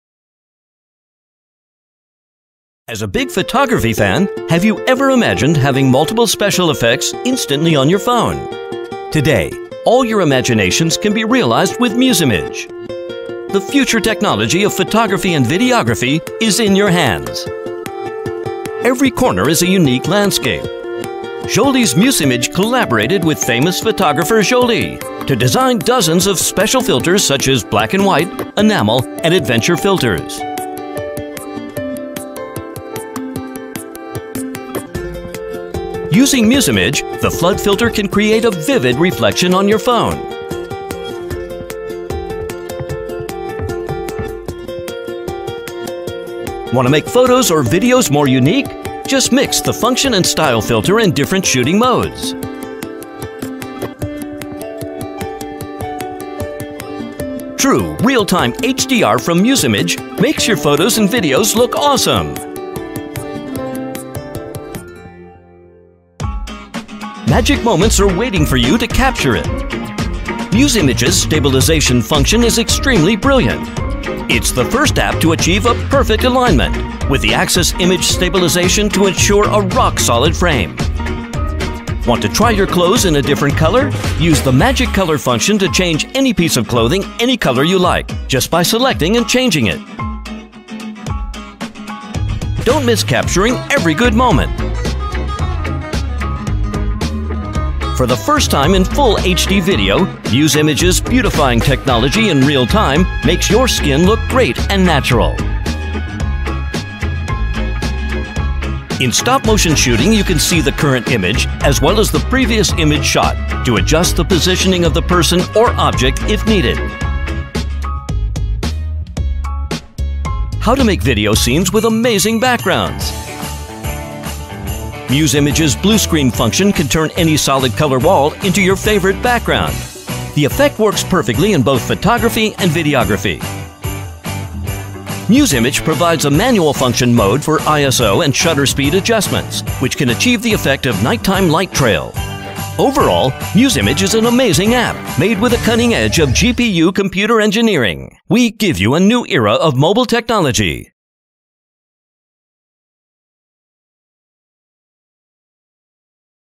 Musemage - The New Era of Mobile Photography
Hello Everyone & Welcome to the New Era of Mobile Photography

I would like to introduce you our revolutionary app called Musemage. Musemage specializes in computational photography. Our core technology focuses on the use of visual computing done on GPU's. We developed a GPU-accelerated application for mobile users to perform incredible photo and video optimizations in real-time while shooting.

You don’t need to do any after editing to the photo and video, it’s all done by using MuseMage. Our apps is developed by a special algorithm and it will do all the job in real time while photo / video shooting.  After you released the button, you get your creative picture or movie!

Musemage is available for download on the App Store now!

Thanks!